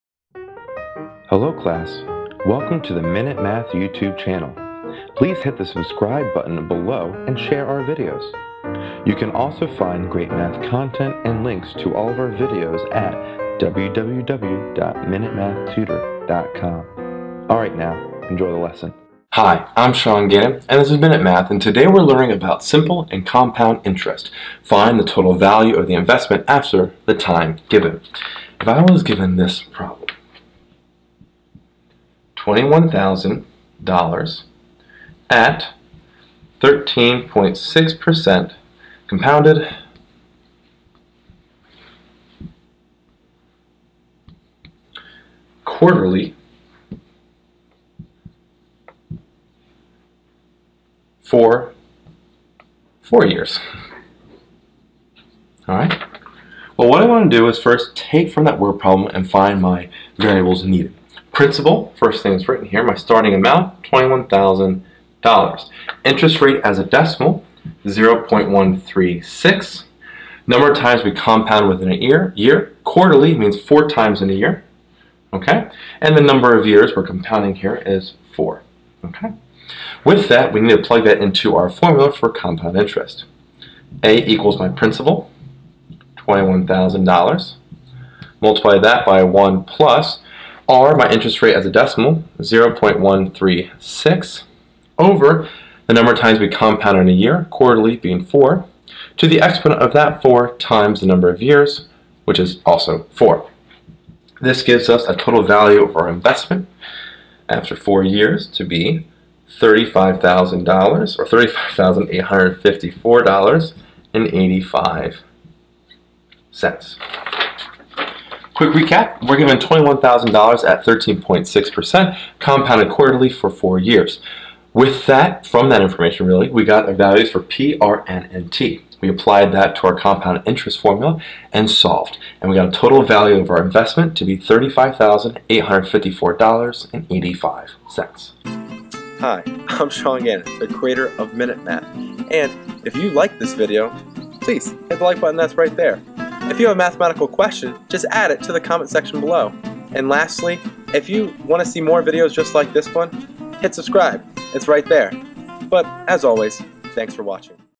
Compound Interest: $21,000 at 13.6% compounded quarterly for 4 years
In this math video lesson on Simple and Compound Interest, I discuss how to use compound interest to find the total investment value of $21,000 at 13.6% compounded quarterly for 4 years.